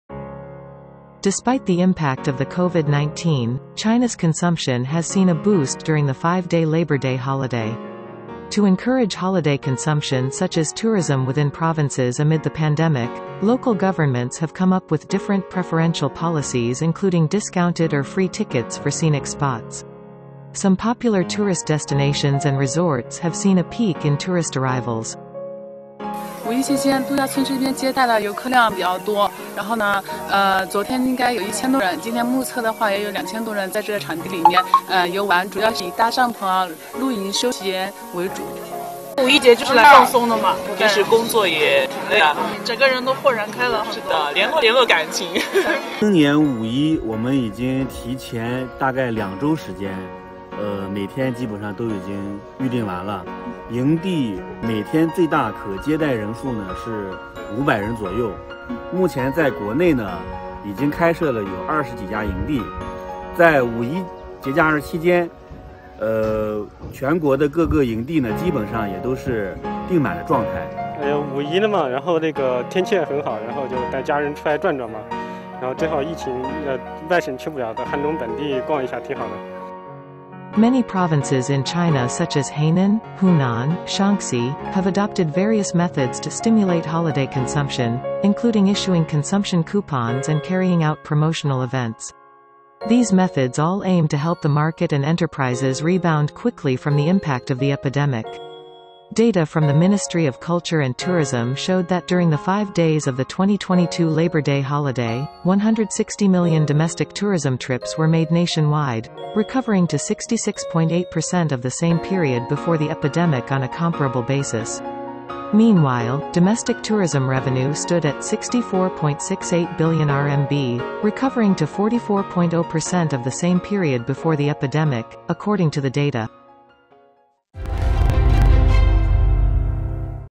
GLOBALink | Holiday consumption trends highlight China's growth potential
Despite the impact of the COVID-19, China's consumption has seen a boost during the five-day Labor Day holiday thanks to various preferential policies adopted by local governments.